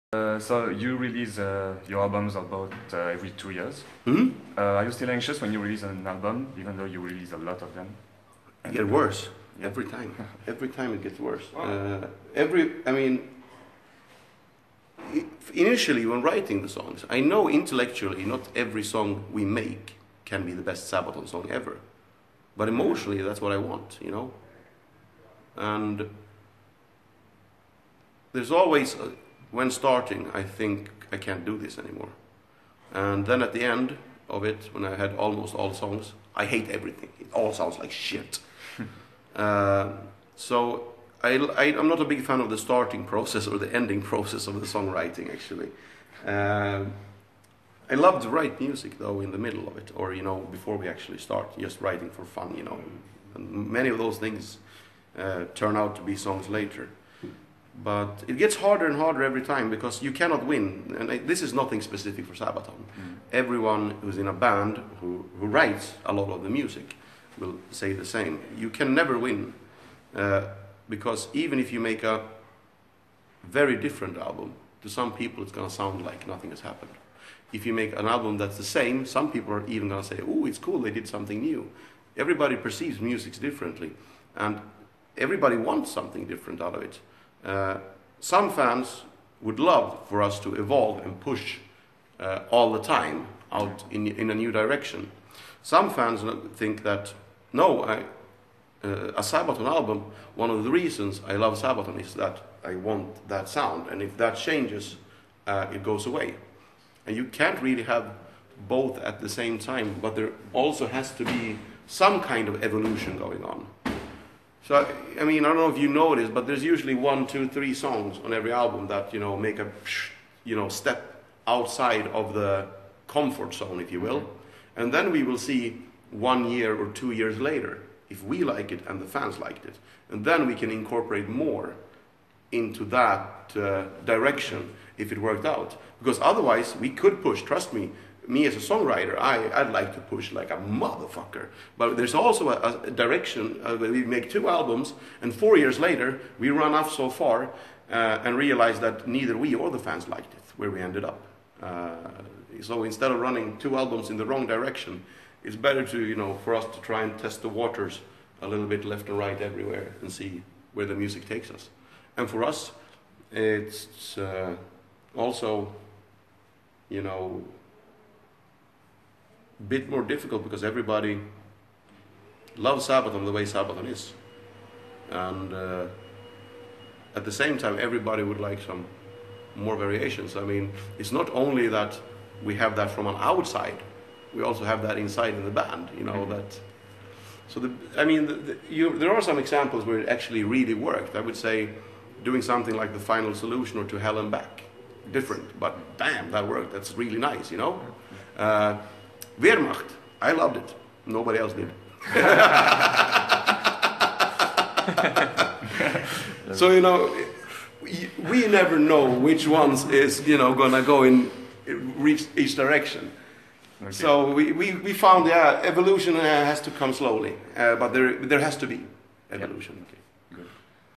Toujours anxieux à la sortie d'un nouvel album ?
Afin de faciliter la lecture de cette longue interview, nous vous proposons d'écouter les enregistrements de quelques réponses.